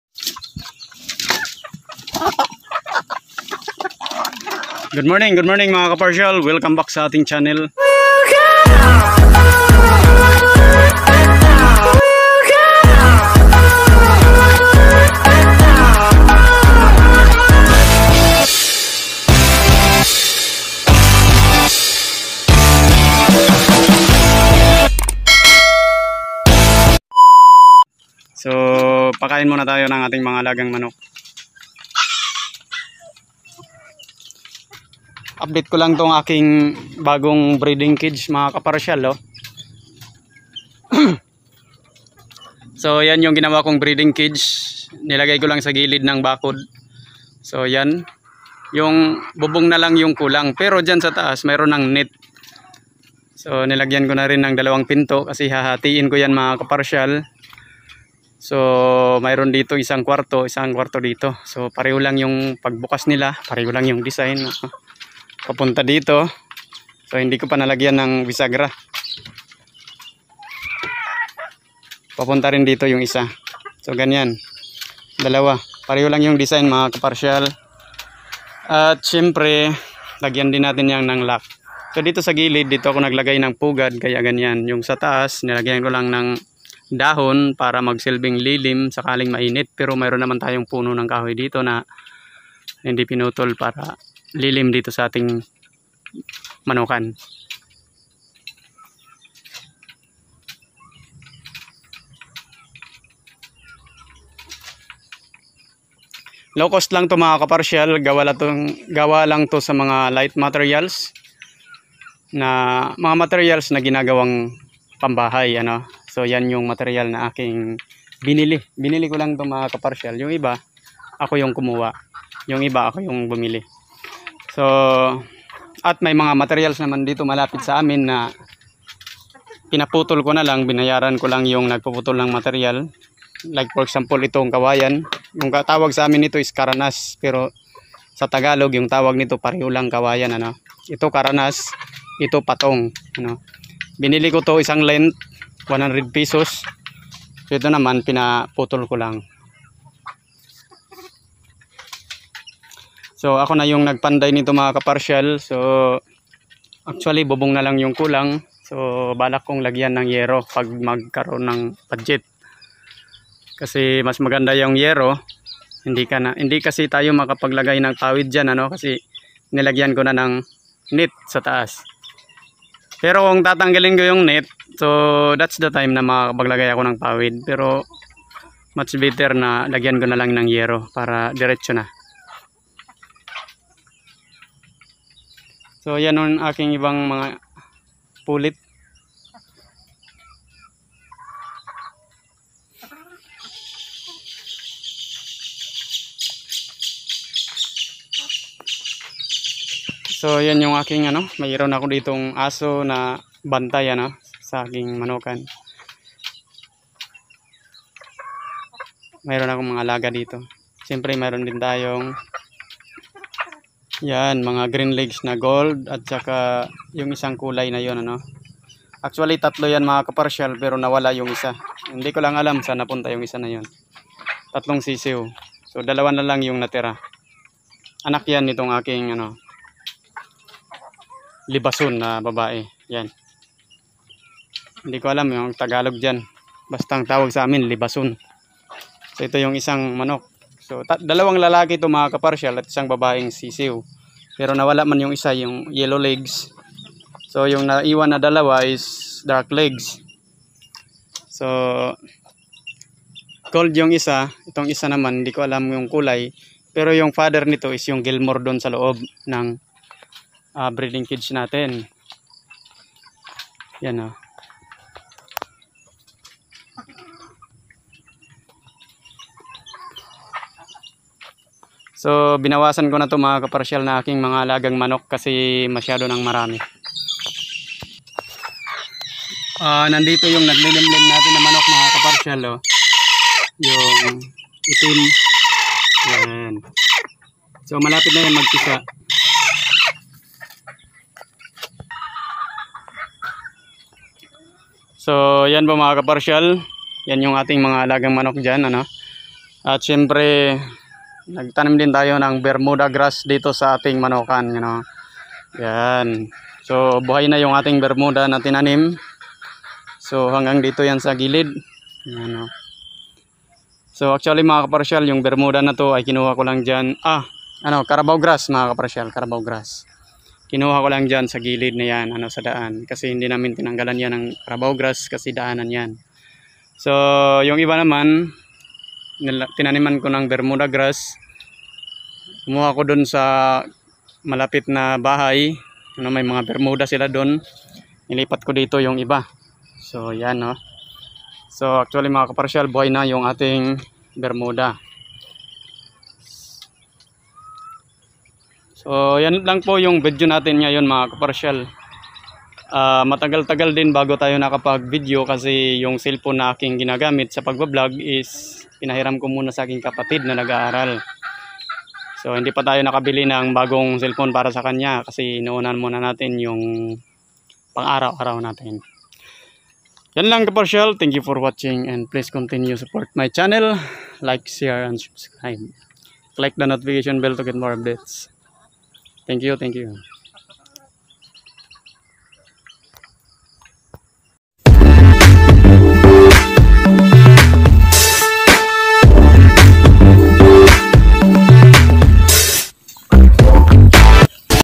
Low cost breeding cage gawa SA light materials + Nakapagtanim din ng Carabao at Bermuda grass
Simple Chicken farming🥚🐣👍

Partialtv